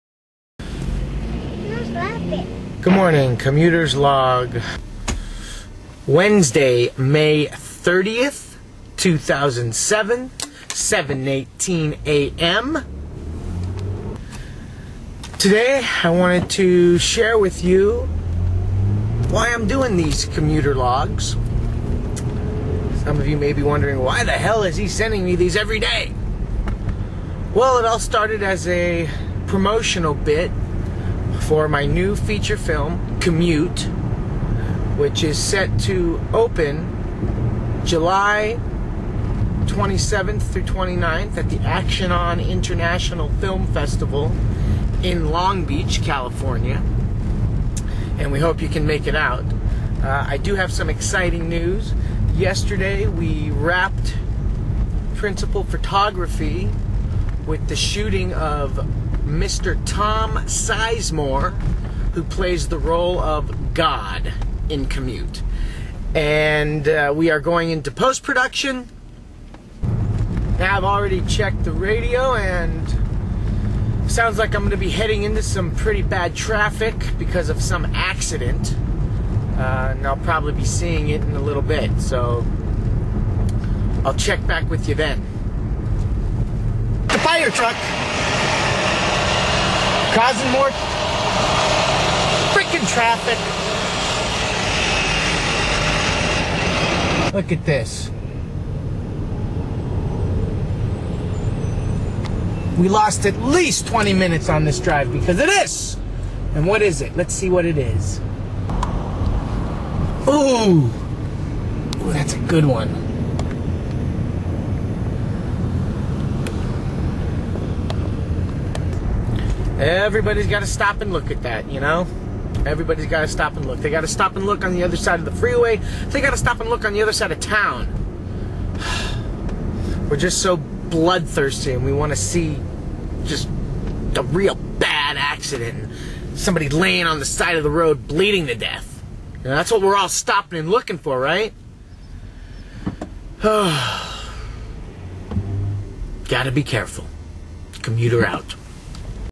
COMMUTERS VLOG MAY 30, 2007
The Commuter explains his VLOGS , promotes COMMUTE and oserves an accident